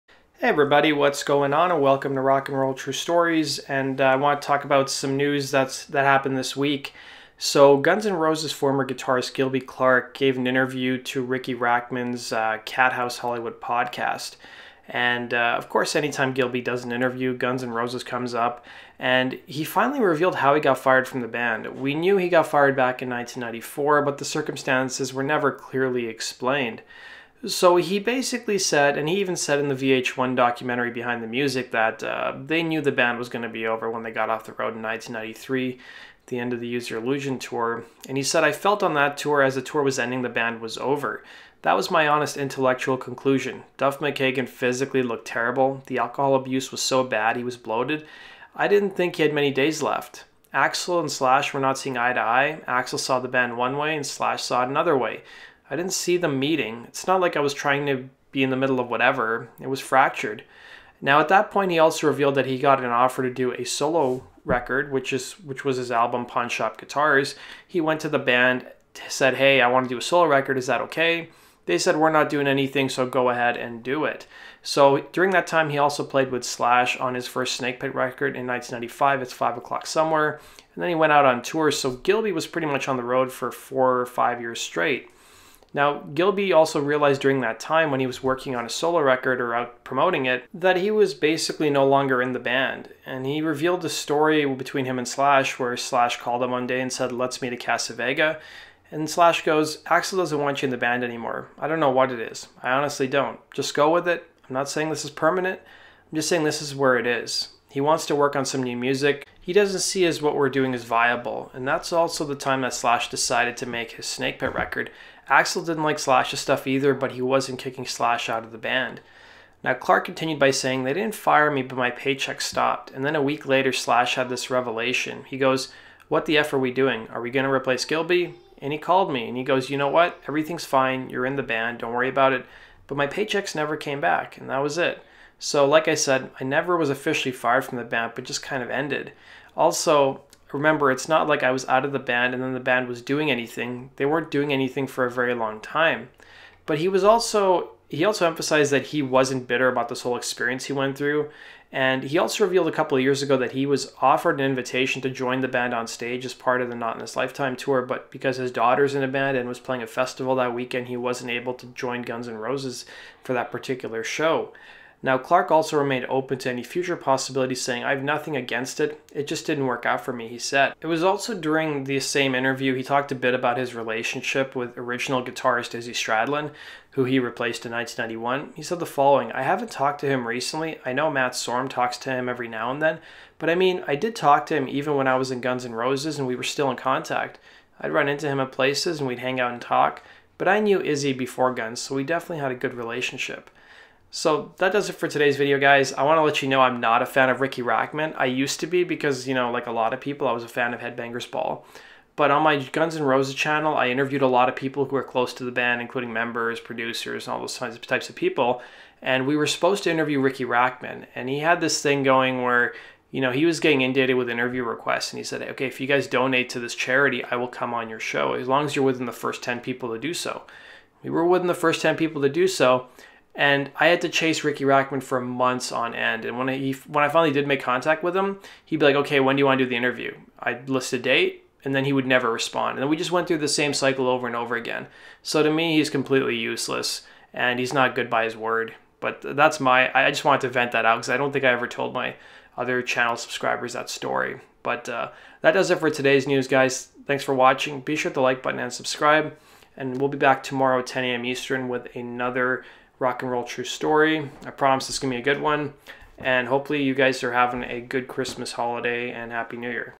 Guns N' Roses Ex Guitarist Gilby Clarke On How He Got Fired & Izzy Stradlin
for the latest rock n' roll news